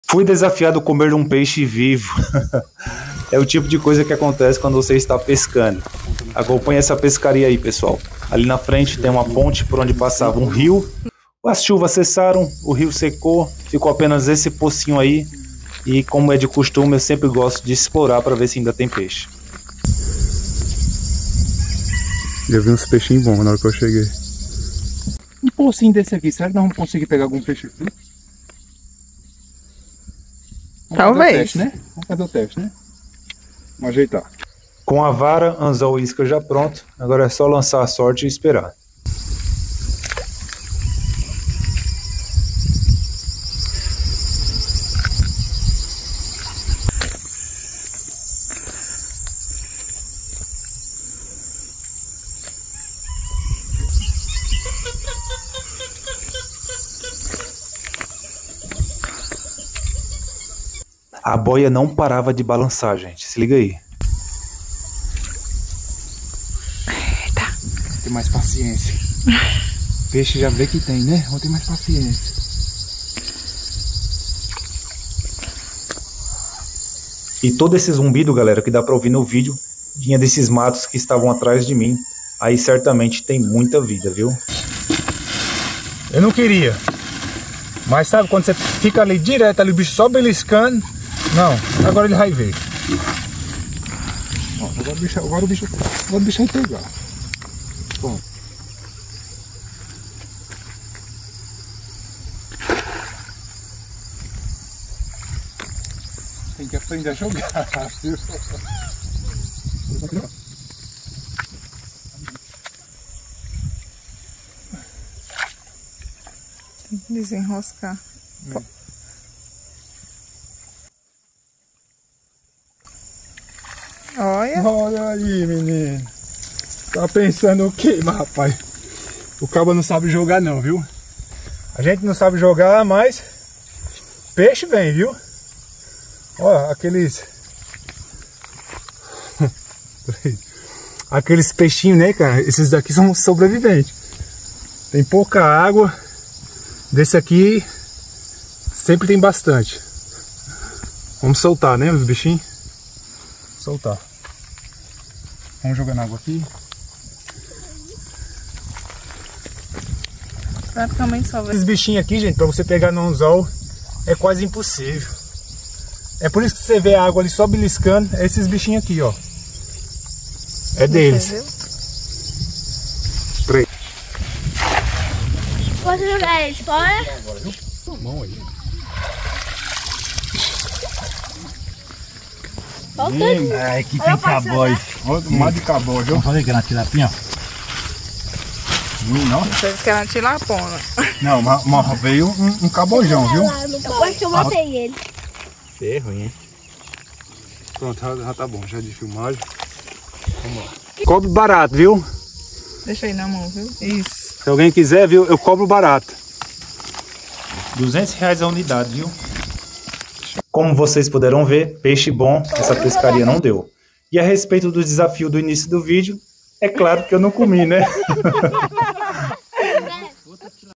COMER PEIXE VIVO É IGUAL COMER SUSHI? DESAFIOS QUE APARECEM EM UMA PESCARIA
Comer peixe vivo é igual comer sushi? Desafios que aparecem quando se vai pescar 😂.